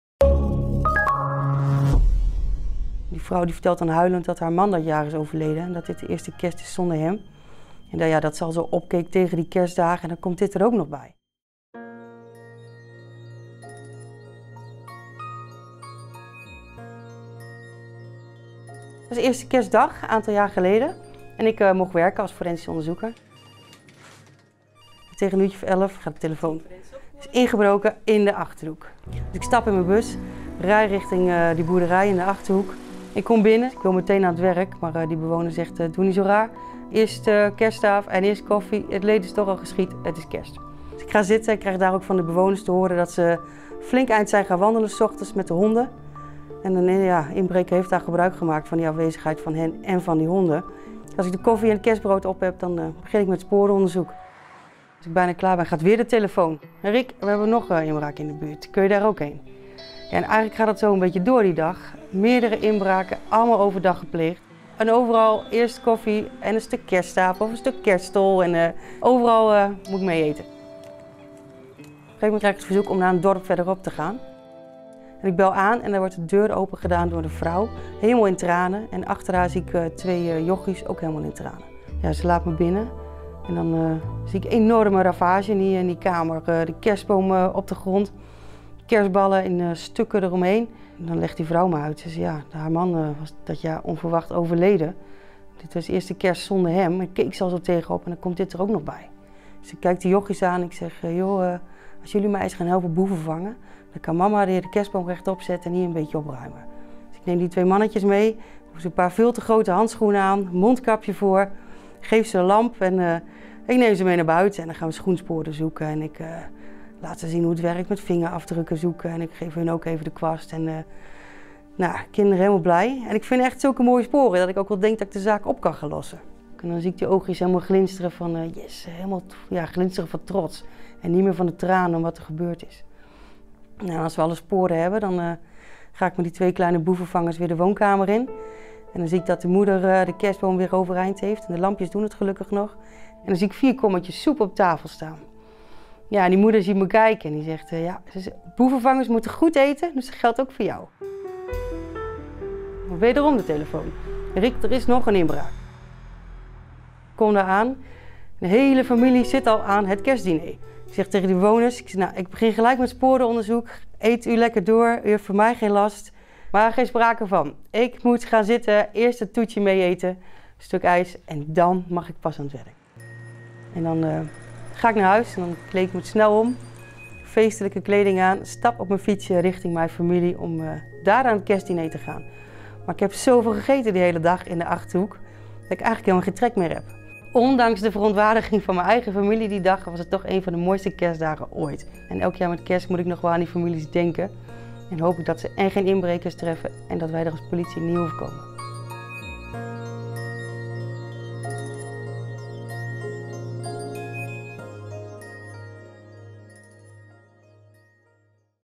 'Kerstdiner' - door Henrieke Schoonekamp - Politieverhaal in beeld
'De vrouw vertelt huilend dat haar man dat jaar is overleden en dat dit de eerste kerst is zonder hem. Dat ze al zo opkeek tegen de kerstdagen en dan komt dit er ook nog bij.'
Henrieke Schoonekamp vertelt over een eerste kerstdag waarop ze bij meerdere mensen sporen moet veilig stellen na woninginbraak.